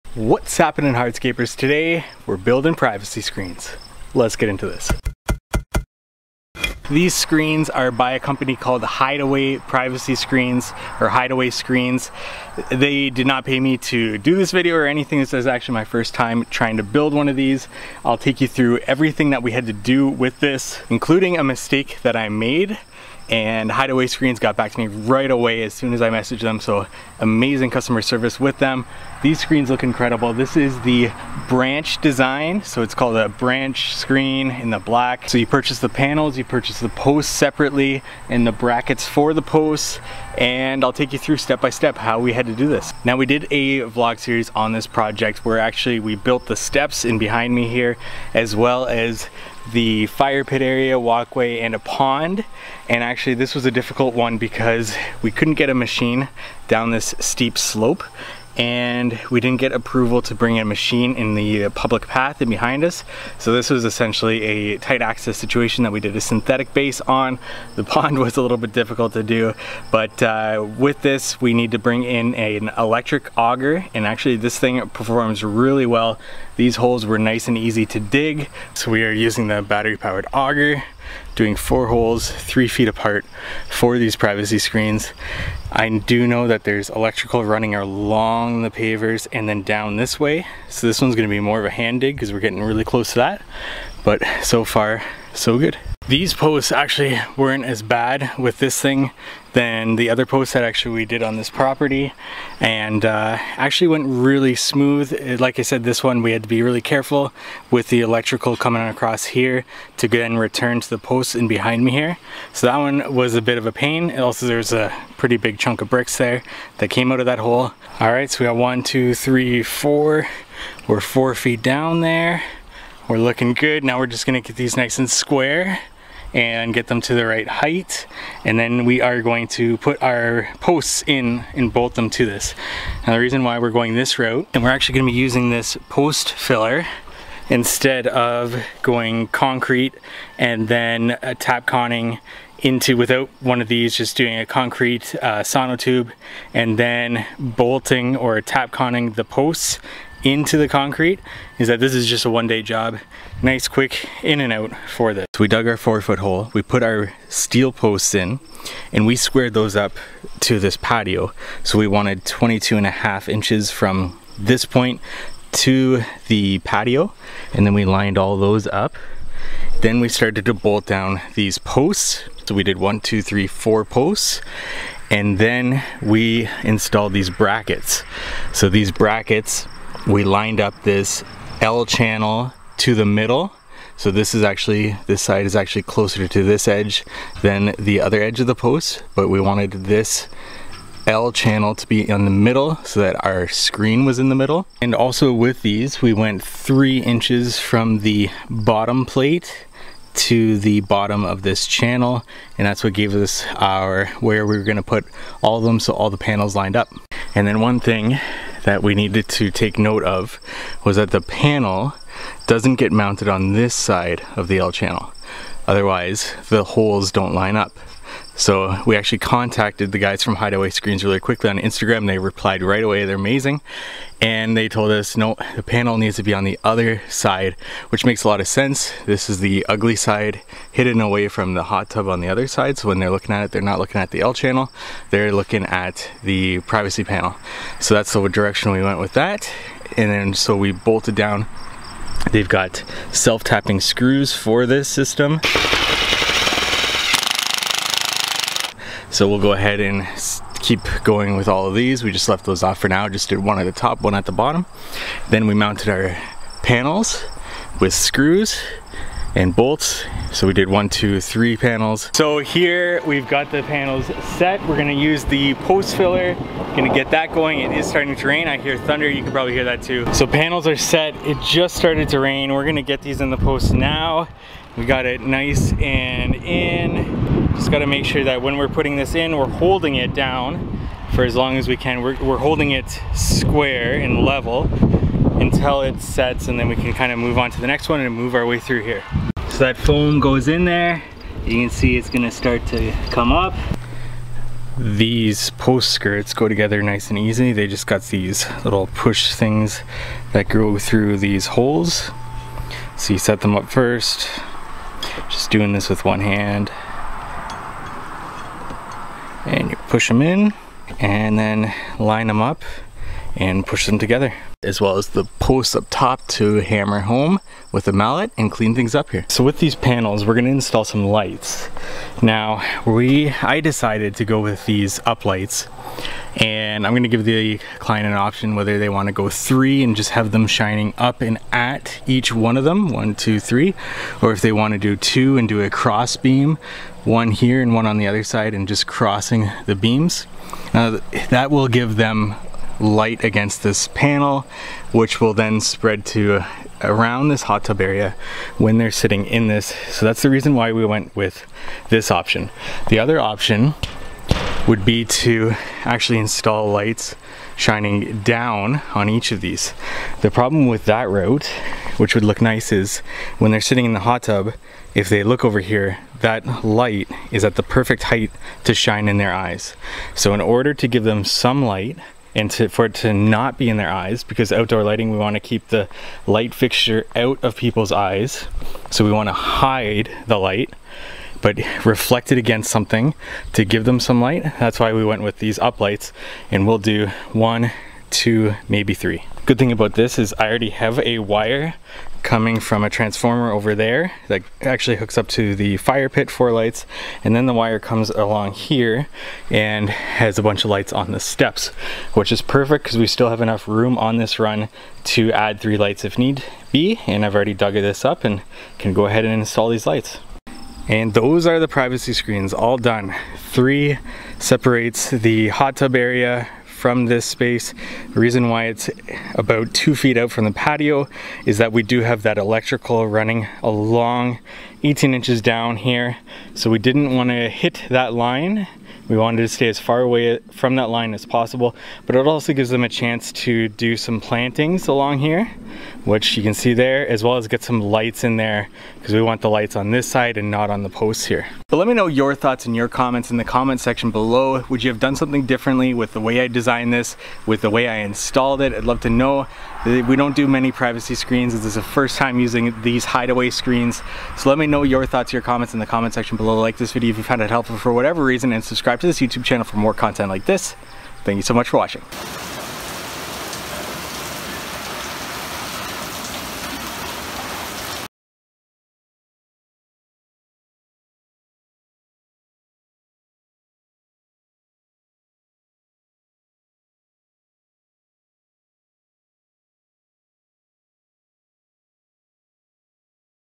How to Install Privacy Screens Easily | Clean, Modern, Lighting
On today's video, we install some privacy screens on a previous project that we did for a client. We walk through step-by-step how we installed them and a problem that we encountered, as well as installing lighting for them.